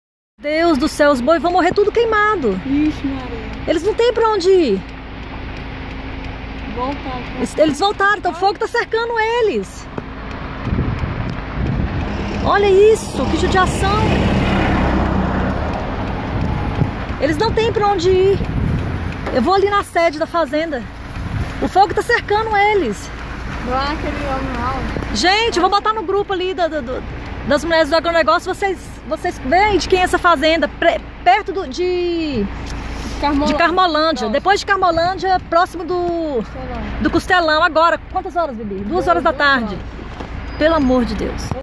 Queimada em Fazenda - Carmolândia
Animais fogem de fogo que consome pastagem seca